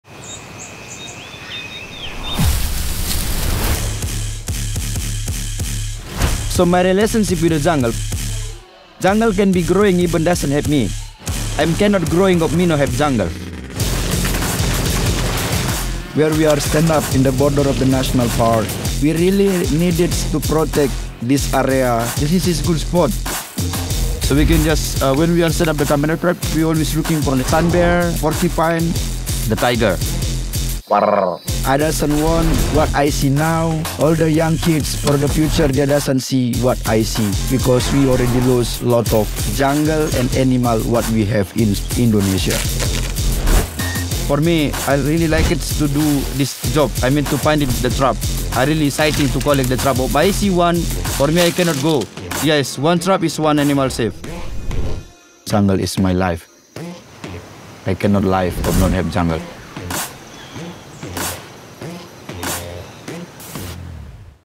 PAM/PUMA BIO/VERSE SS23 — Environmental Impact
For it’s second season the BIO/VERSE has funded a series of environmental impact actions in the buffer-zones of the Gunung Leuser National Park, Indonesia, in partnership with PUMA, P.A.M., and DEEP.

A community-led action program built with Sumatran Ranger Project supports jungle conservation strategies, ranger development and patrols, snare removal, conflict mitigation, community education and community infrastructure.

To support research and habitat management efforts, and combat illegal activity like logging and poaching, a series of wireless devices have been deployed by Rainforest Connection to record and transmit the sounds of the jungle for analysis by global scientific communities.